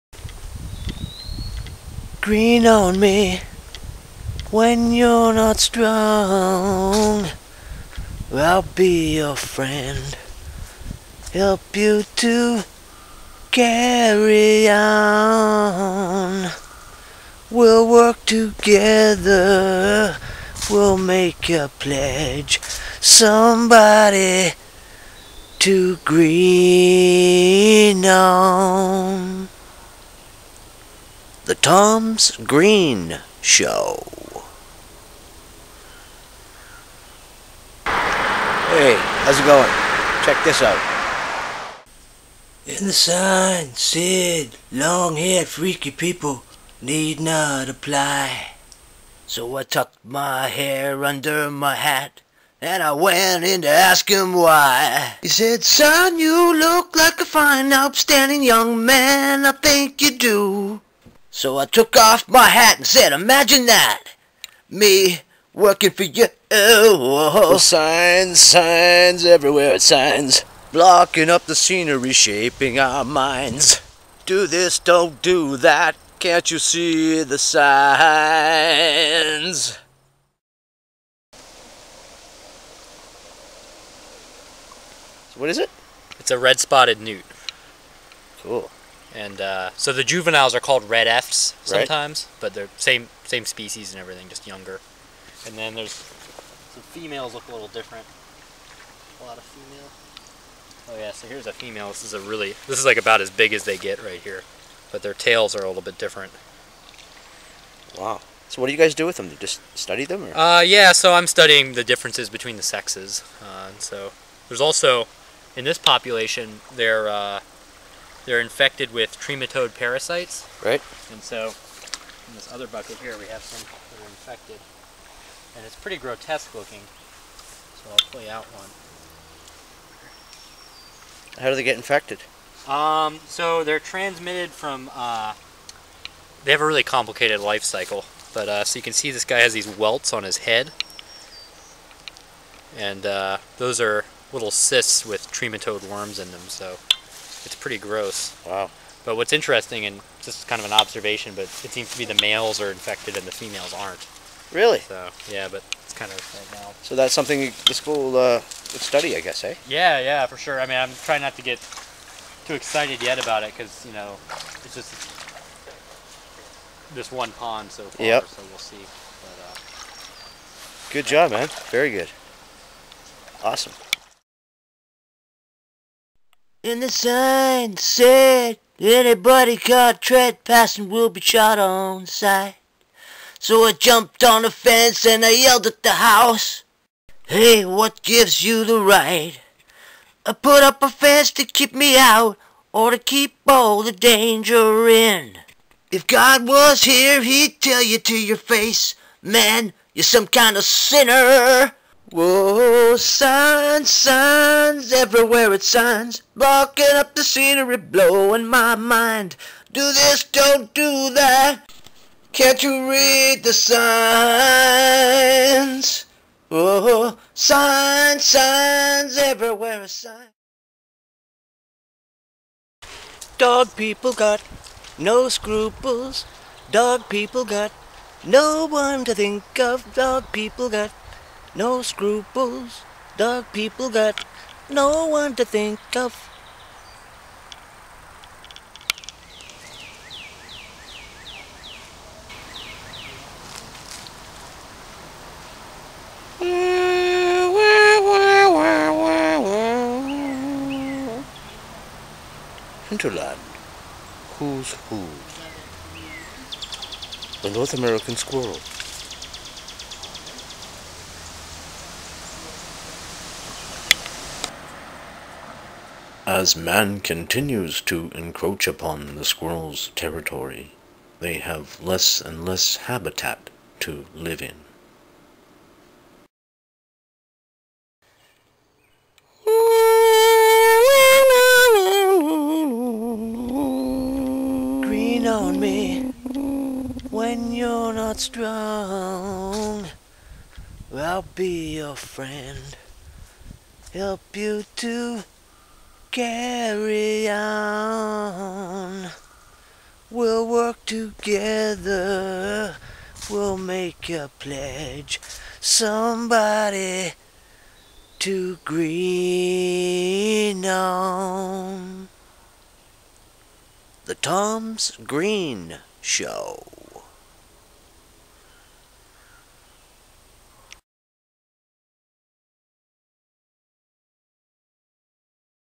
The Tom's Green Show Episode 1
Host Tom Pearson looks at anything green connected. In episode one a play on signs via song, a visit to a York Region pond where some male water dwellers have developed odd growths on their heads, Hinterland Who's Who spoof, and not so green dog walkers.